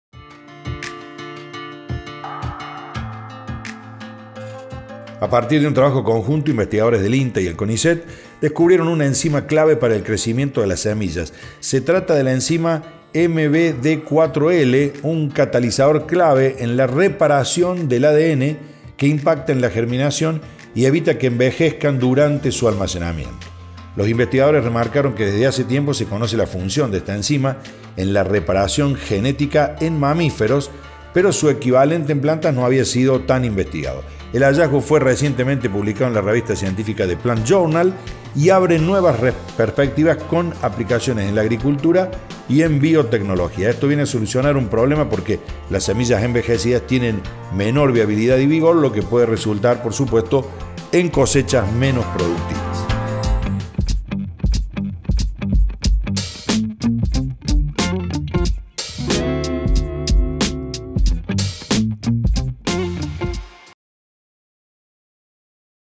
ENZIMAS PARA LA REPARACION DEL ADN
A partir de un trabajo conjunto, investigadores del INTA y el Conicet descubrieron una enzima clave para el crecimiento de las semillas. Se trata de la enzima MBD4L, un catalizador clave en la reparación del ADN que impacta en la germinación y evita que envejezcan durante su almacenamiento. Los investigadores remarcaron que desde hace tiempo se conoce la función de esta enzima en la reparación genética en mamíferos, pero su equivalente en plantas no ha sido tan investigado. El hallazgo fue recientemente publicado en la revista científica The Plant Journal y abre nuevas perspectivas con aplicaciones en la agricultura y en la biotecnología.  “Esto viene a solucionar  un problema, porque las semillas envejecidas tienen menor viabilidad y vigor, lo que puede resultar en cosechas menos productivas”.